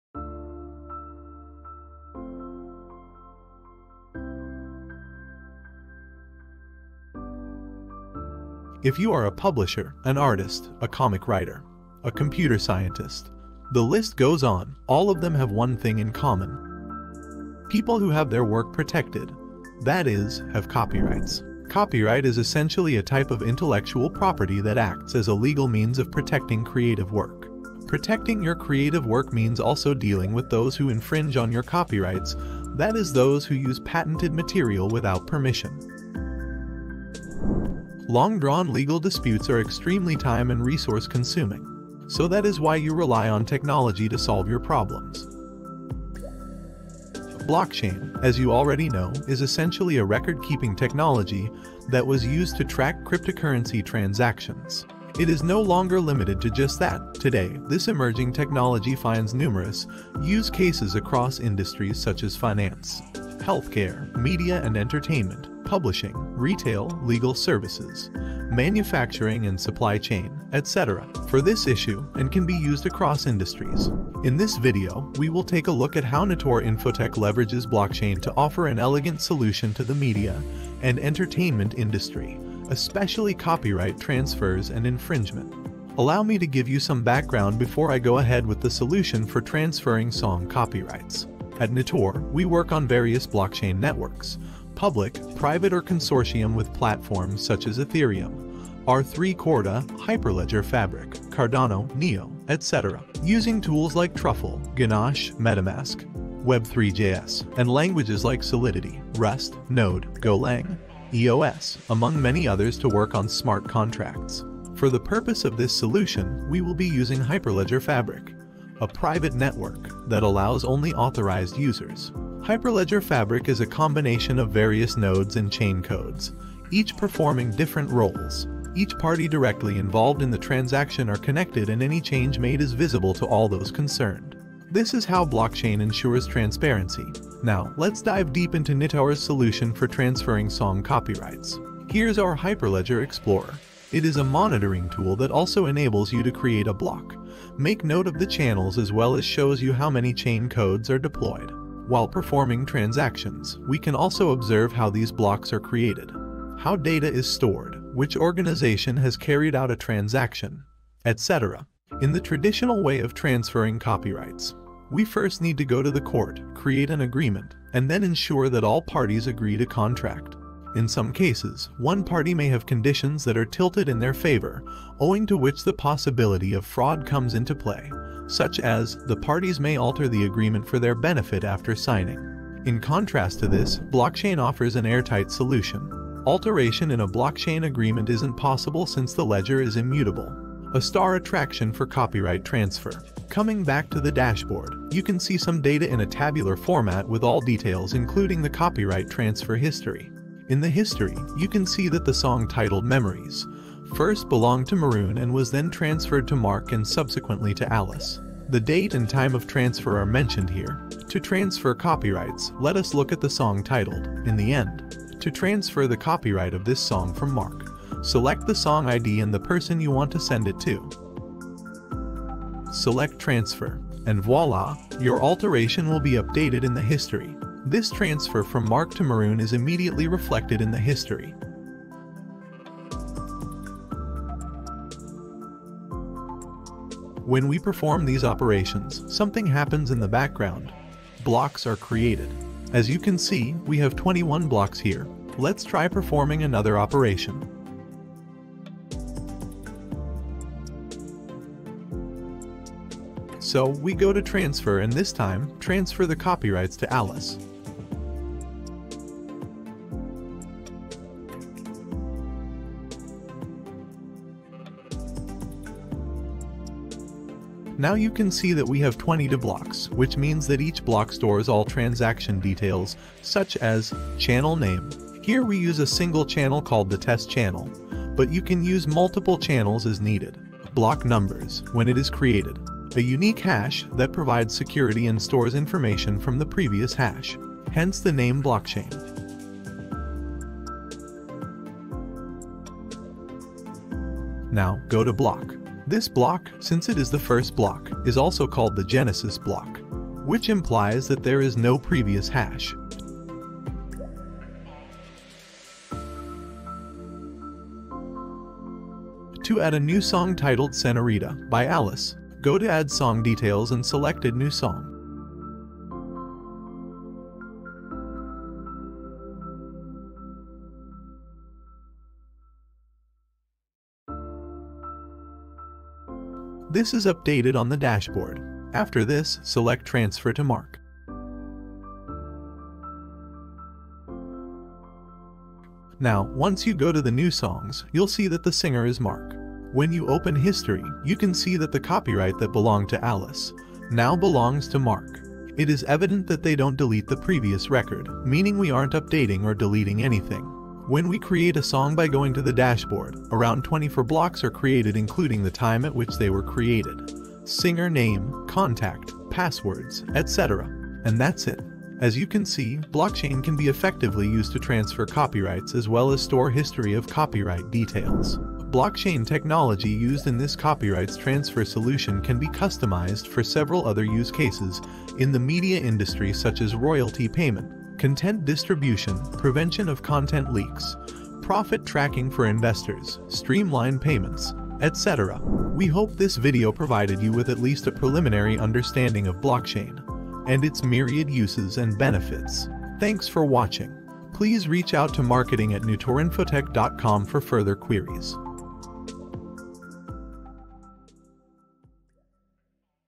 Blockchain For Song Copyright
Copyright can help you keep your data safe and secure from fraudulent activities. Blockchain is a record-keeping technology that helps you track your activities across various industries like media, healthcare, supply chain, etc.

We helped the media and entertainment industry copyright transfers and infringement with blockchain. Watch this video to learn about Nitor Infotech’s blockchain capabilities and see how you can ensure transparency while transferring song copyrights.

to learn more about our services.